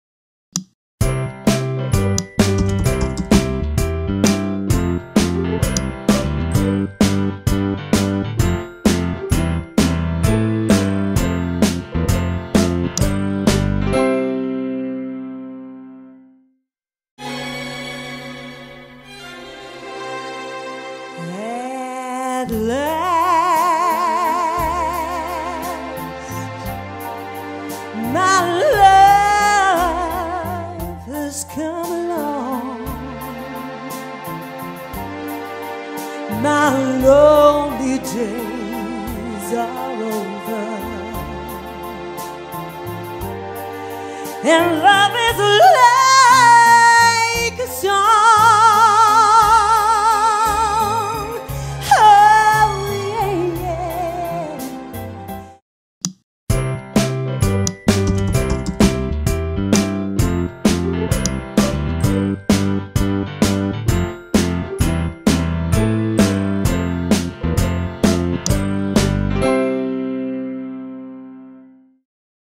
ConnectionsEnts Joanna Lee
Joanna Lee available for weddings, corporate events, clubs or shows, this amazing female vocalist performs an array of classic songs from all era's. see more and book for your next event